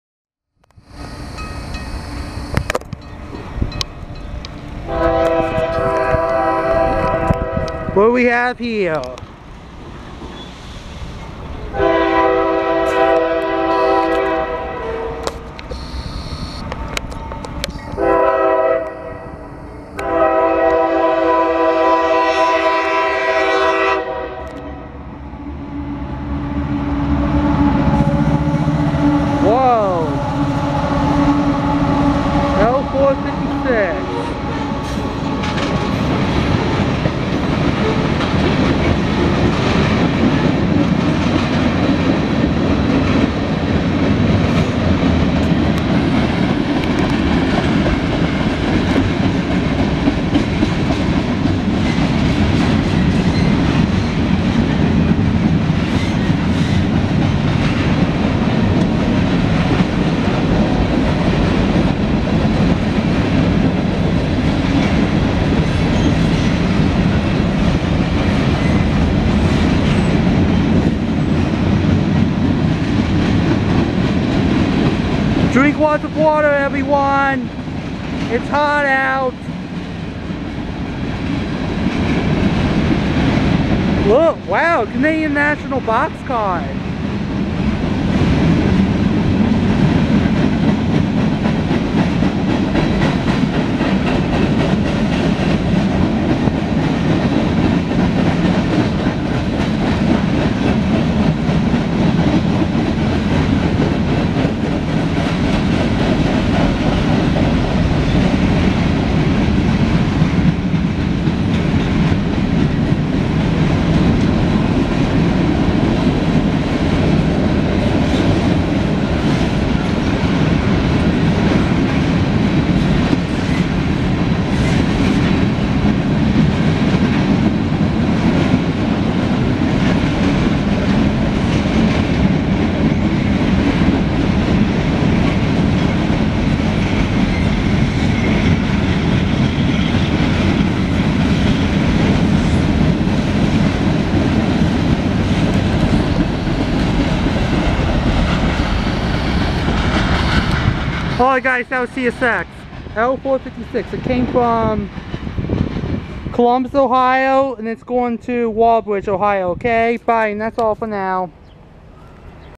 (8-28-2024) CSX northbound local mixed freight train in Marion Ohio.
This is a CSX northbound local mixed freight train in Marion Ohio and the symbol for the local mixed freight train in this train video was CSX L456 and CSX L456 came from Columbus Ohio and it went to Walbridge Ohio.